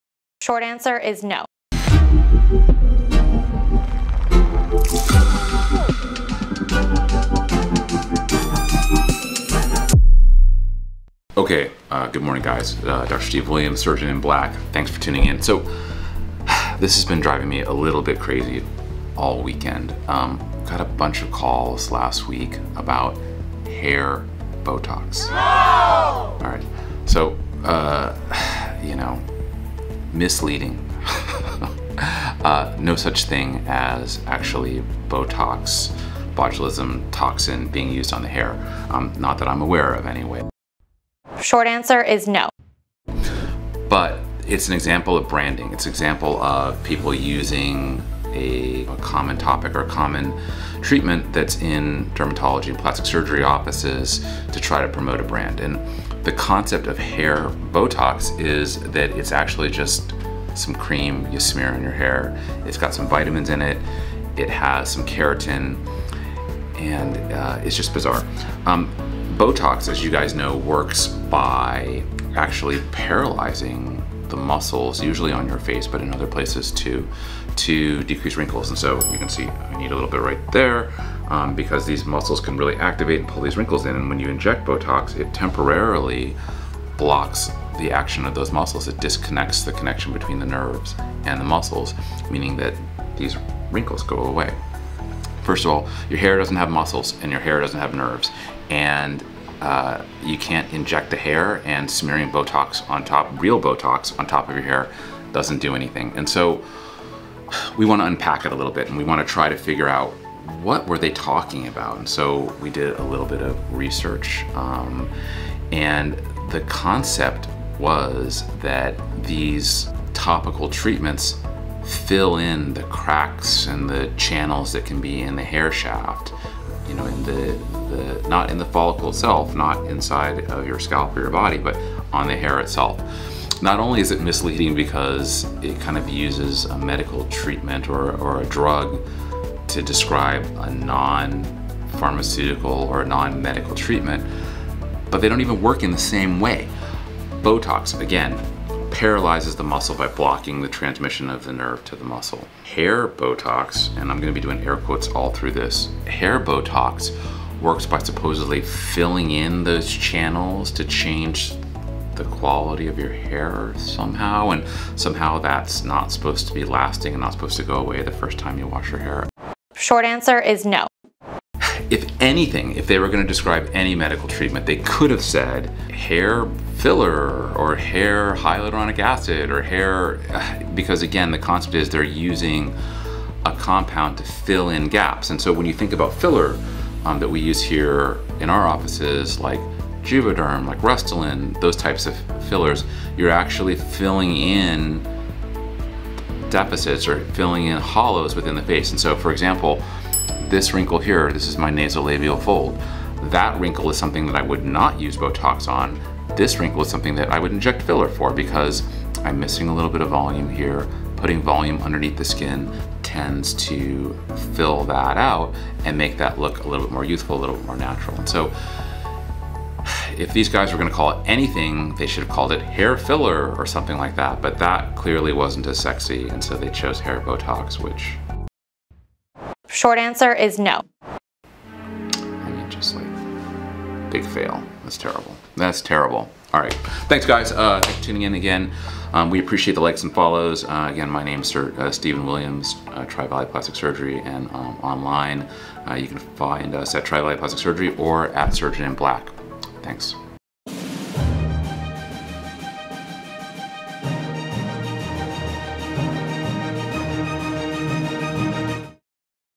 What is Hair Botox?
Is that even real? According to Jasmin, the short answer is NO! Hair Botox is a hair treatment that has nothing to do with real botox. This product shares a name with botox for the sake of brand recognition, but shares zero similarities. Join Dr. Williams as he reveals the truth about hair botox. Be sure to watch till the end as we debunk this misleading product.